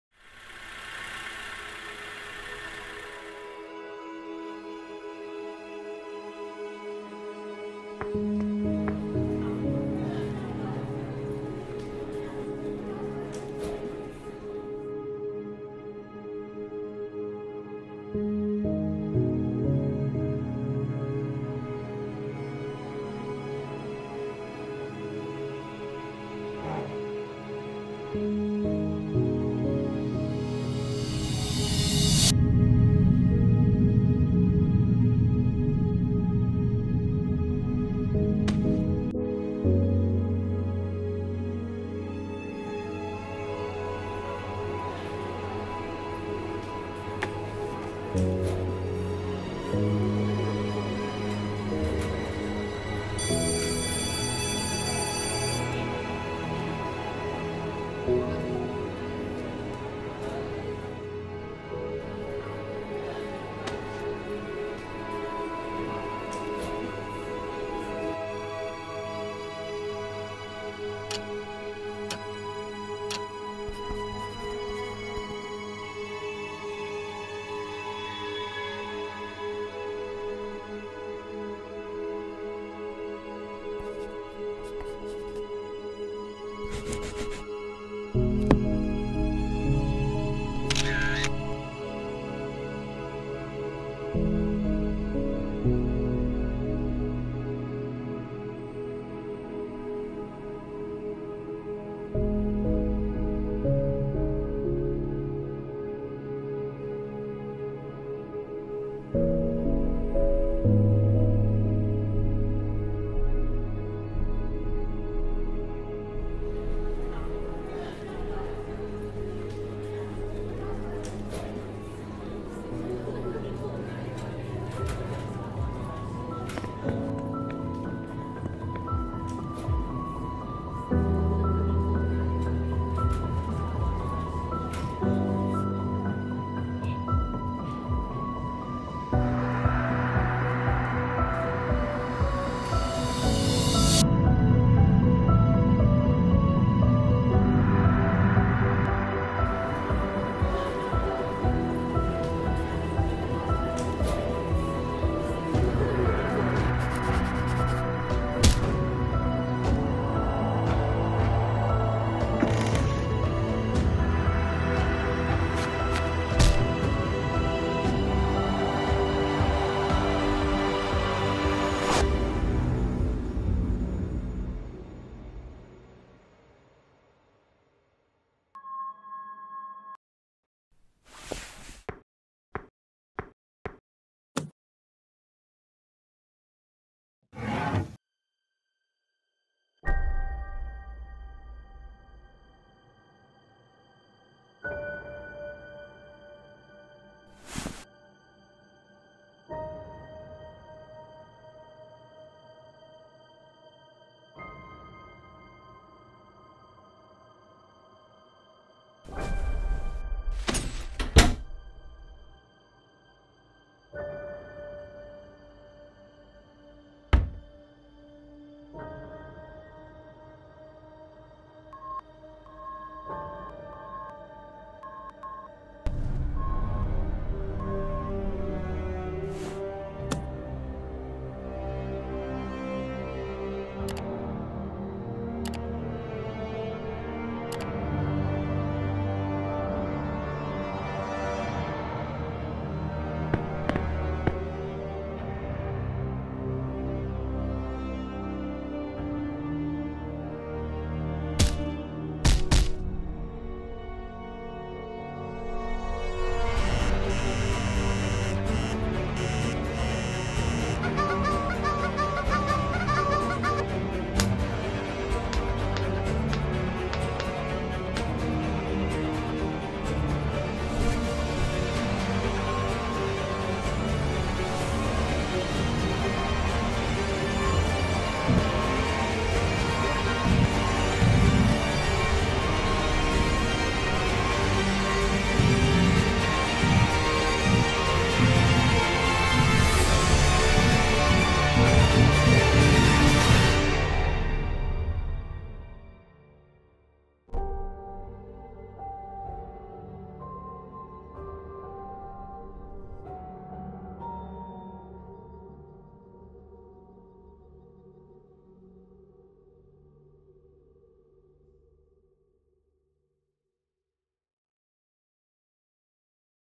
Gas (Carbon Monoxide) Poisoning - Indie animated film
A long and big animation project I did for fun by myself, cuz I just love showing something depressing and suicidal lol :)
I hope the few of you, liked this short animation film, and I hope I can make more in the future if it's something you guys like! :D

Auuugghh... This was a huge cringe to watch. I had so much fun making this, but at the same time, rewatching it makes me wanna delete it lol.

BTW I've decided to add some cryptography, so u guys can solve out the names of the 2 girls :) (there is actually some lore behind this too so ya)

And save chinchillas!